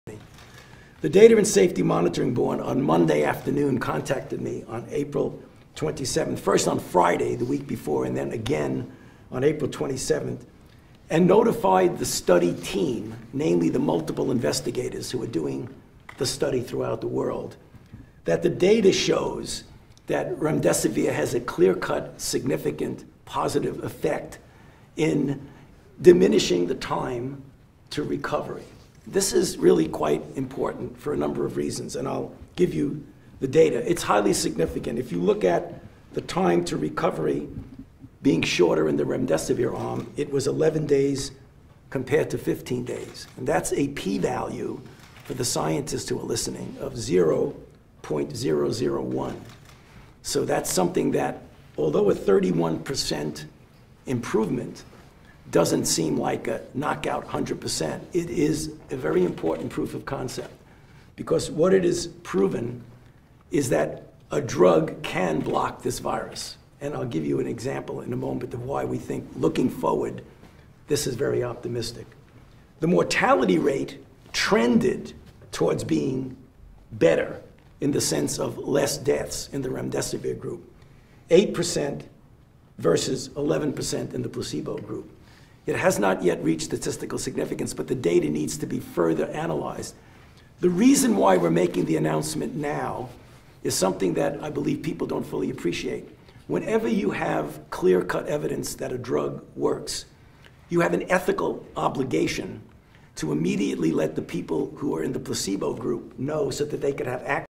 Dr. Fauci “Clear Cut Evidence That Drug Works” Remdesivir
Dr. Antony Fauci on  Remdesivir Drug Study “Good News”.